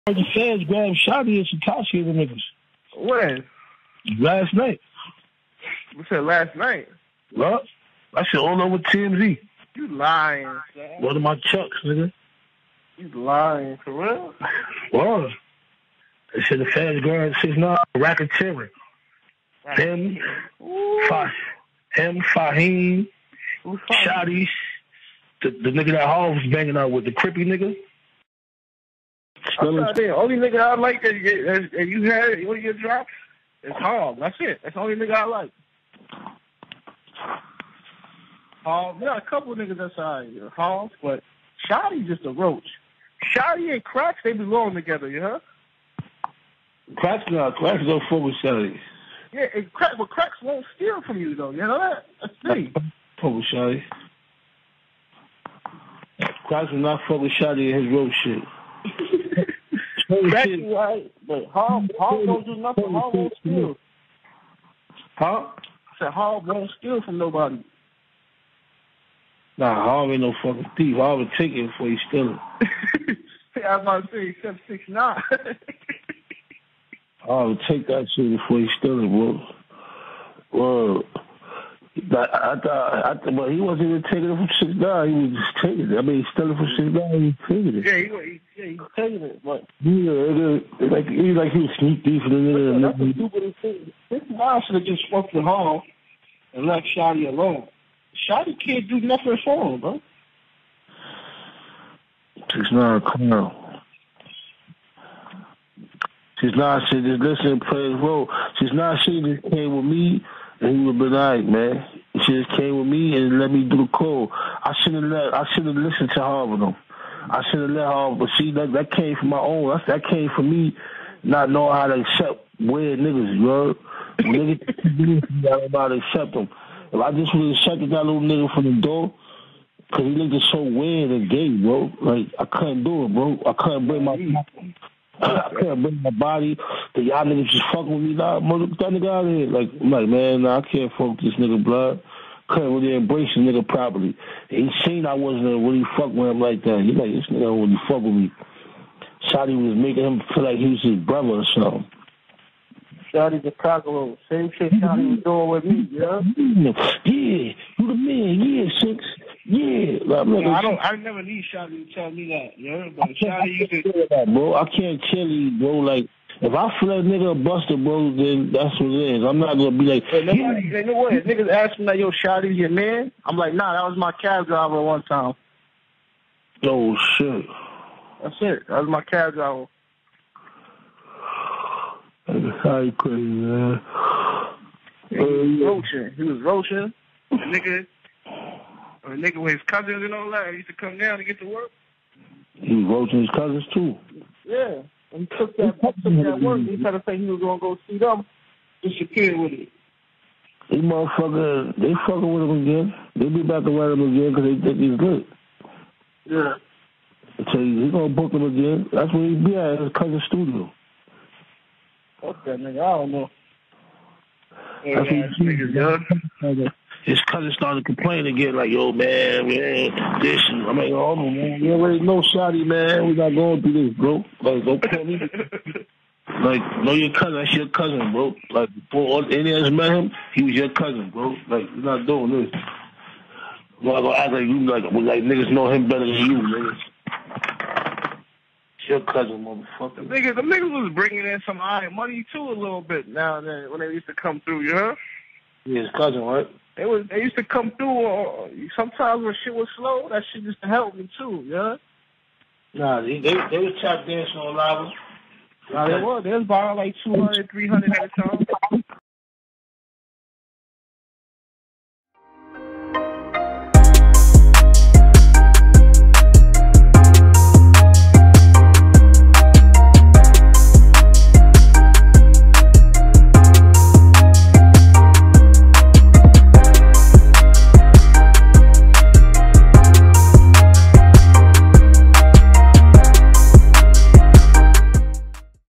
Mel Murda And CEO Kris Dissing "Shotti" & "Tekashi 6ix9ine" On FBI Wiretap "He's A Roach" !!!
CEO Kris, who confirmed he was working with the federal government, was one of the first people Mel Murda called when Shotti and Tekashi 6ix9ine were first arrested for the RICO case back in November.

Unfortunately for Mel Murda, he was on the line with an informant, who helped bring down the Tr3yway family of Brooklyn and Harlem.

Take a listen to the conversation above, where they praise Harv as the only member they both respected, as well as Mel Murda believing he should’ve never let Shotti manage Tekashi 6ix9ine, who Kris called a "roach."

If you are not satisfied when your video appears here.